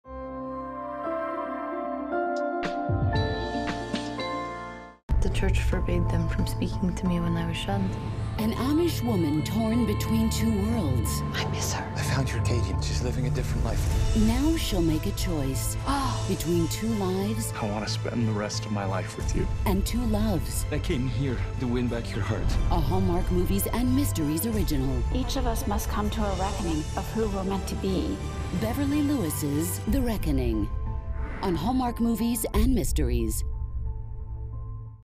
Beverly Lewis' The Reckoning
Katie Mayfield is now the mistress of Mayfield Manor. In her new world she finds herself torn between two worlds and grieves for her plain Amish life. Does Katie choose her new life or the simple heritage she left behind?